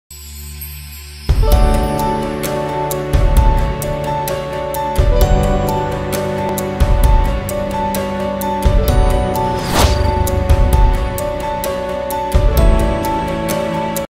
Blue Butterfly Pendant Necklace with Crystals
The white gold plated necklace and an elegant Butterfly-shaped crystal from Austria complete the design. It is not only pleasing to the eye, but also used to convey the emotions of beauty, happiness and hope. This butterfly crystal bracelet will be your favorite.